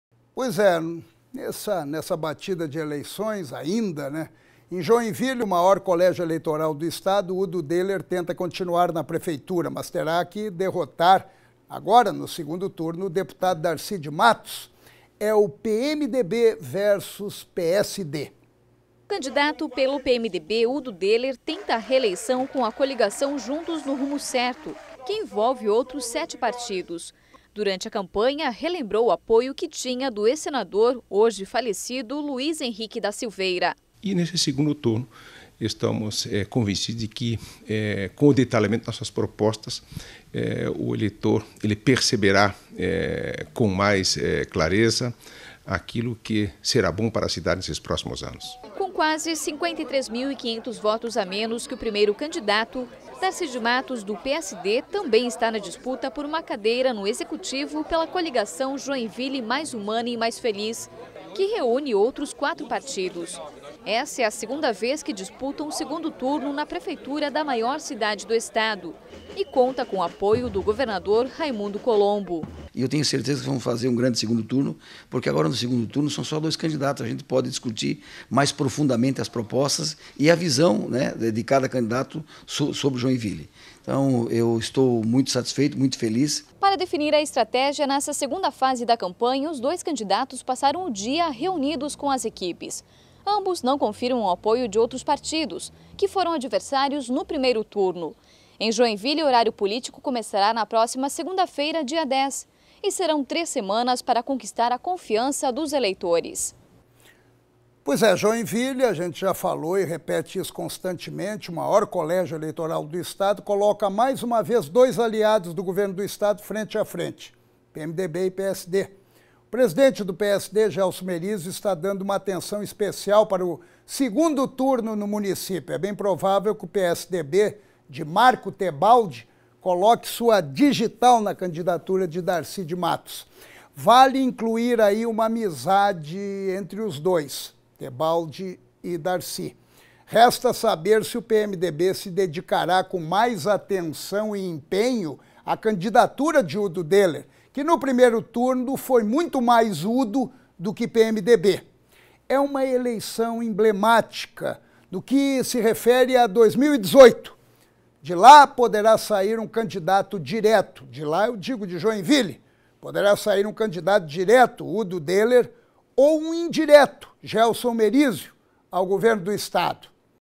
Segundo turno em Joinville
Em Joinville, maior colégio eleitoral do Estado, Udo  Dohler tenta continuar na prefeitura, mas terá que derrotar no segundo turno o deputado Darci de Mattos, PMDB versus PSD. A repórter Dani Lando traz mais informações sobre essa disputa, diretamente de Joinville.